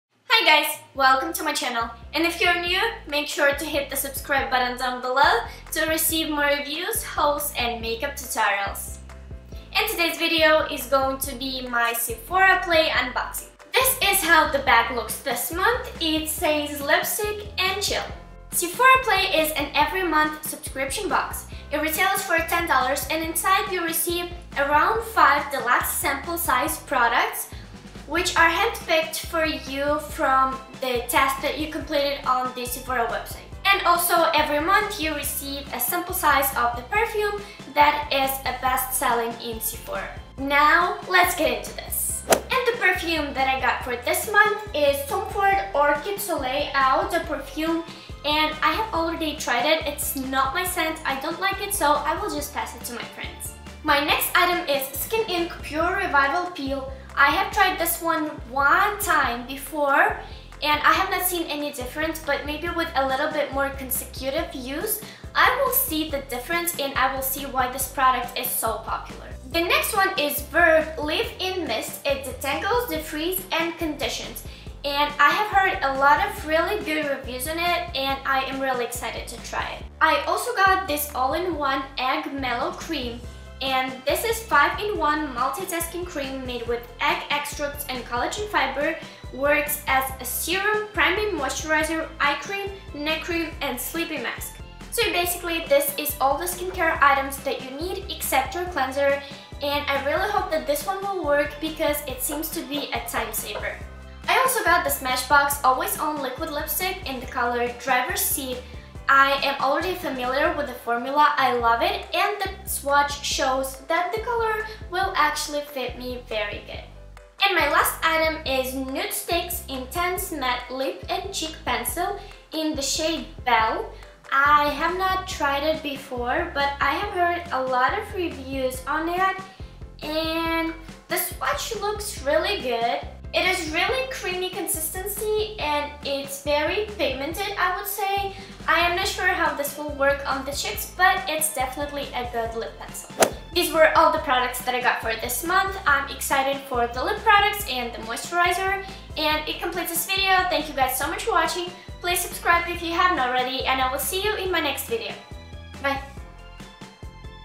Sephora Play Unboxing October 2016
Hi guys! (WATCH IN HD)
Check out what I got in my box this month!

Todays look:
Eyes: Studiomakeup On-The-Go Eyeshadow Palette, ELF Mineral Infused Mascara
Face: L'oreal Infallible Glow Foundation,  Too Faced Sun Bunny Bronzer, L'oreal True Match Blush in Baby Blossom, Too Faced Candleglow highlighter in Warm Glow
Lips: Too Faced Melted Liquid Lipstick in Chihuahua